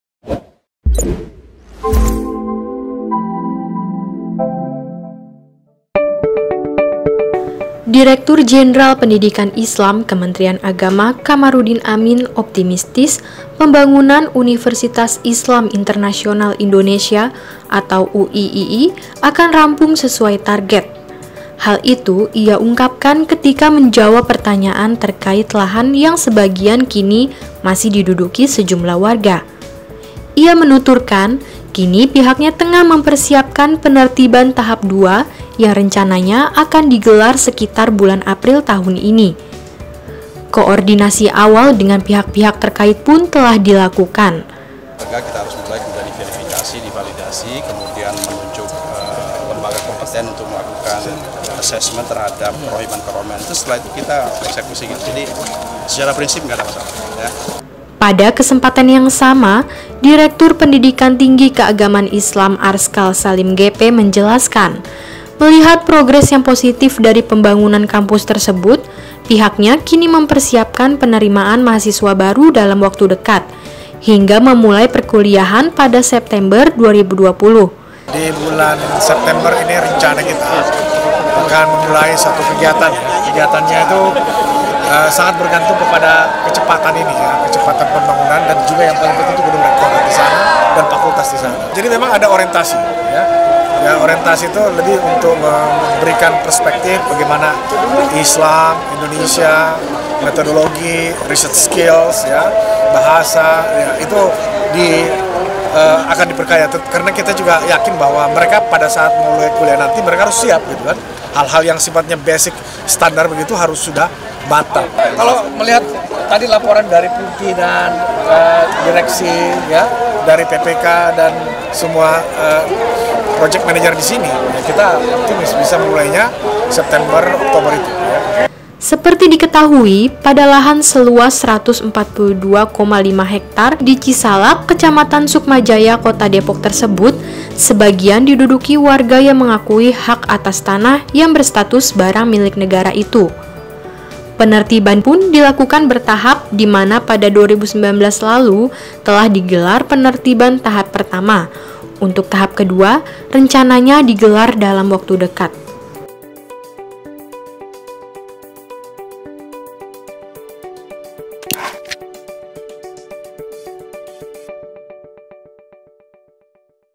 Kemenag Optimistis Kampus UIII Dapat Beroperasi Sesuai Target
Dirjen Pendidikan Islam, Kamaruddin Amin bersama Direktur Pendidikan Tinggi Keagamaan Islam, Arskal Salim GP mendampingi Menteri Agama RI, Fachrul Razi dan Mantan Wakil Presiden RI, Jusuf Kalla meninjau lokasi proyek Pembangunan Kampus Universitas Islam Internasional Indonesia, di Cisalak, Kota Depok, Jumat (24/1).